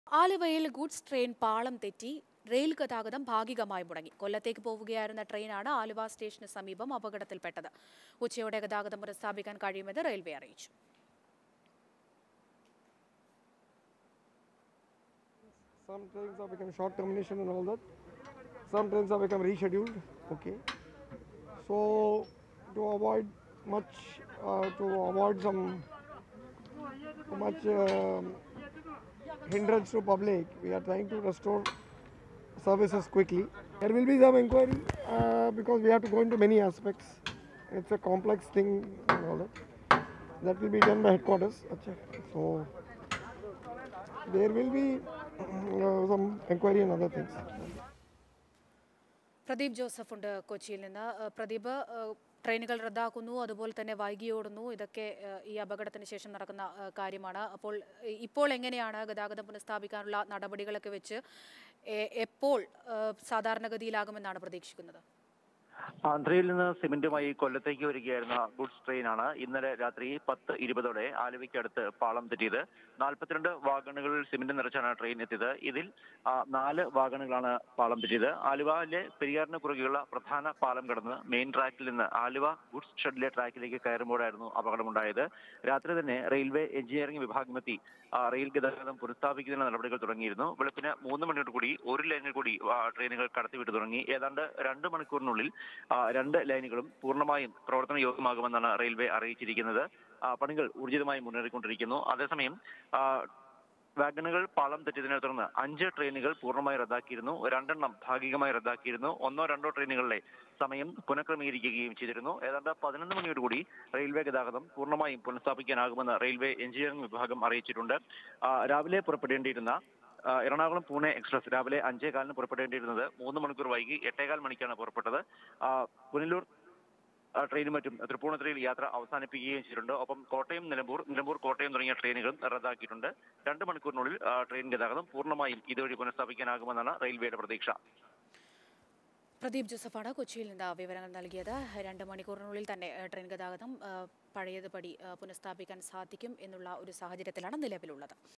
ആലുവയിൽ ഗുഡ്സ് ട്രെയിൻ പാളം തെറ്റി; റെയിൽ‍ ഗതാഗതം ഭാഗികമായി മുടങ്ങി| Mathrubhumi News
ആലുവയിൽ ഗുഡ്സ് ട്രെയിൻ പാളം തെറ്റി റെയിൽ‍ ഗതാഗതം ഭാഗികമായി മുടങ്ങി.

Happy viewing!